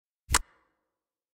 Click - Free To Use Sound Effects
Click effect. A "click" sound effect is typically a short, sharp sound that resembles the action of pressing a button or a small mechanical device being triggered. It's often used to represent the act of selecting an option in digital interfaces, activating small switches, or as an indication of a light, swift connection being made or broken. The sound is crisp and has a very brief duration, usually no more than a fraction of a second. It's characterized by a clear, high-pitched 'tick' or 'click' noise, which can be subtle or pronounced depending on the context and the type of device or interface it's associated with. This sound is commonly found in various applications, from software user interfaces to physical devices like mouse clicks or camera shutters.